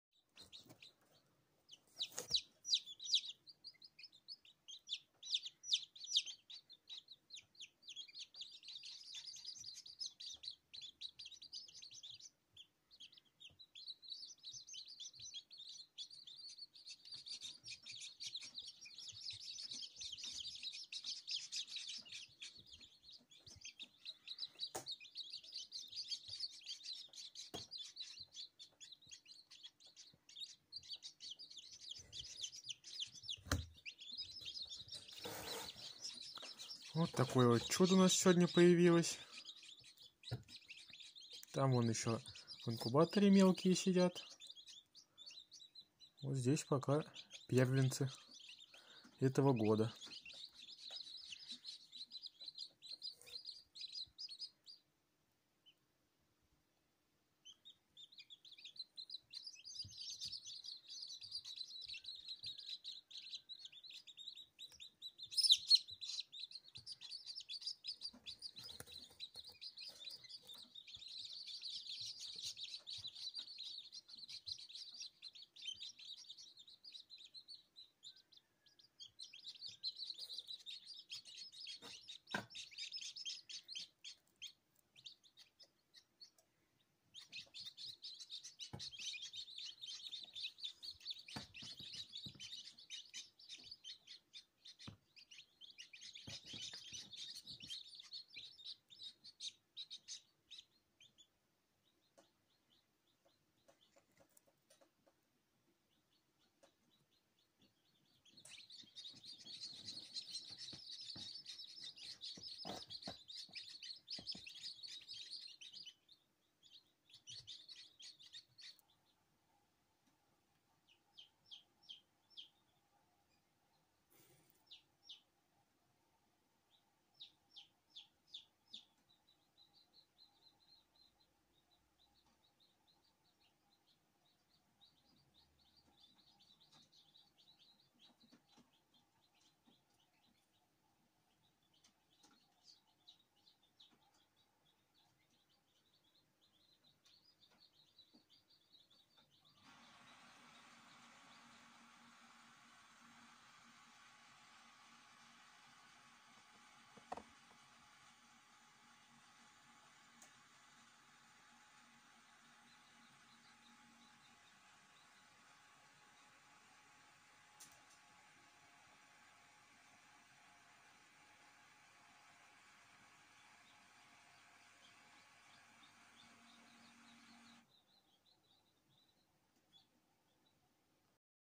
Первый вывод в этом году.
Вывод 2.04.20. Инкубатор «Несушка-63» из 33 яиц
4 неоплода и вывелось   цыплят